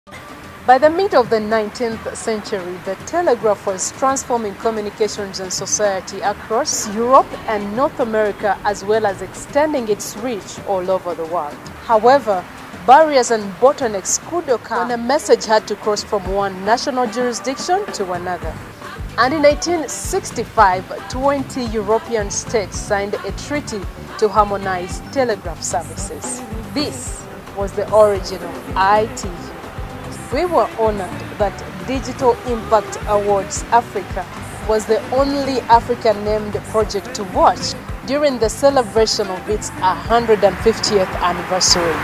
HiPipo ITU Achievement | Digital and Financial Inclusion
HiPipo ITU Achievement - The origin of ITU | Digital and Financial Inclusion, here is the true origin story for International Telecommunication Union